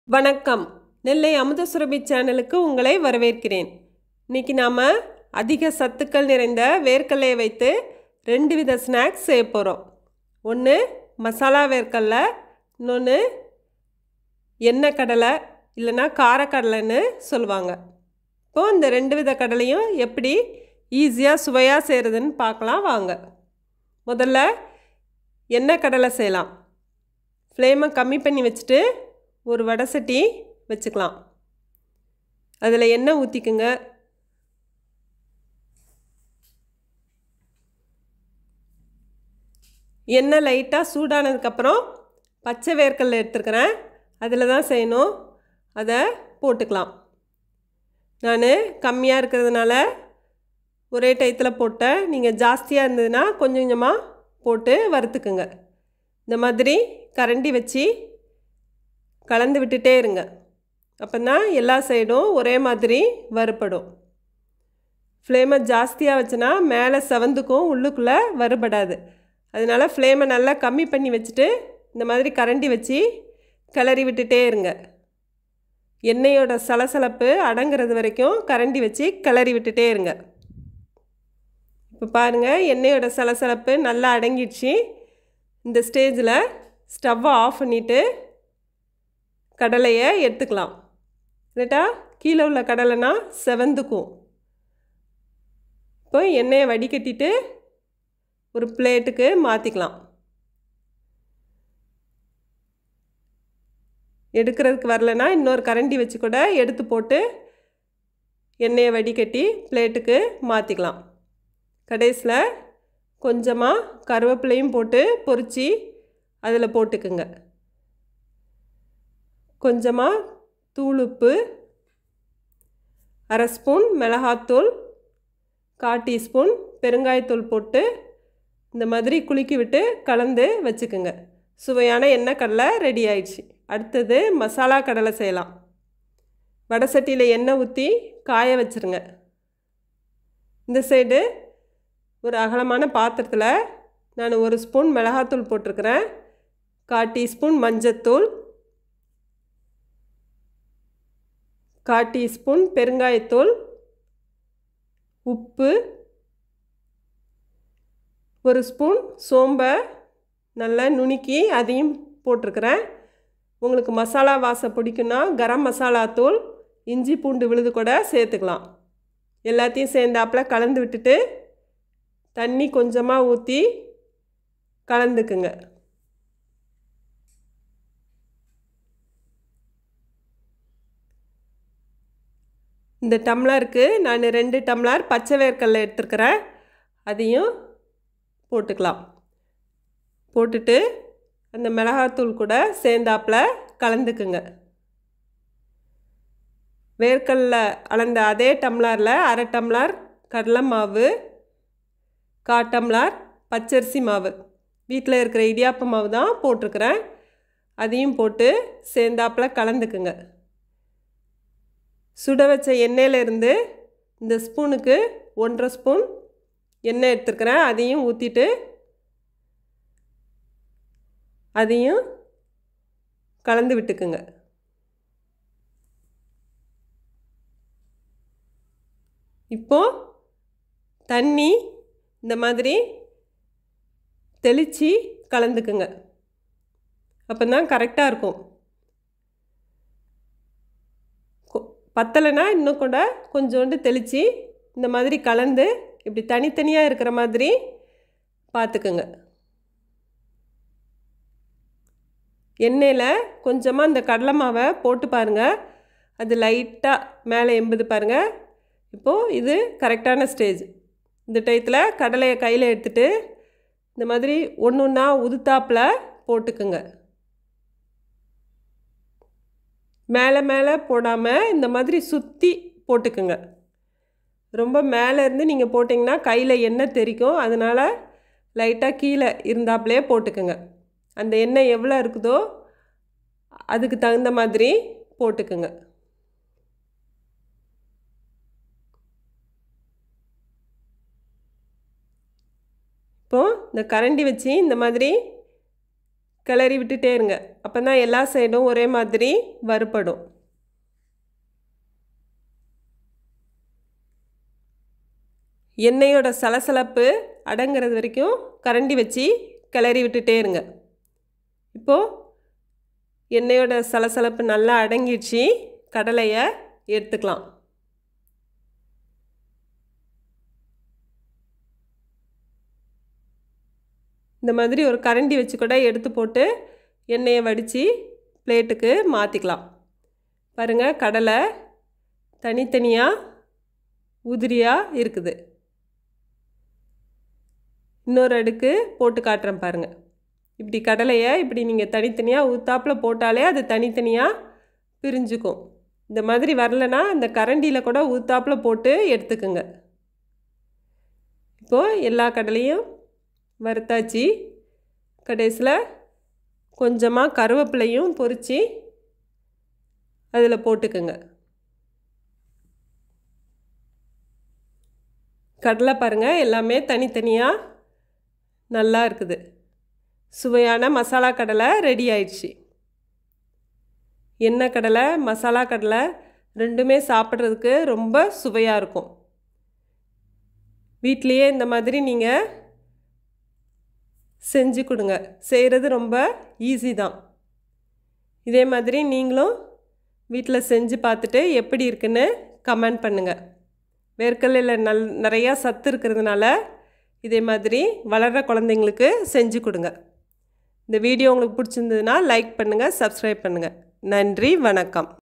Masala kadalai in Tamil/Masala peanut in Tamil/Kara Kadalai in Tamil/Peanut Snack Recipes in Tamil
Welcome to Nellai Amudha Surabi Channel.
This video is about how to prepare masala kadalai and kara kadalai in tamil.

Try this easy and tasty kadalai recipe. Share this recipe to your friends and relatives. Also share your feedback about this recipe in comment box.

Recipe 21: Masala kadalai, Kara kadalai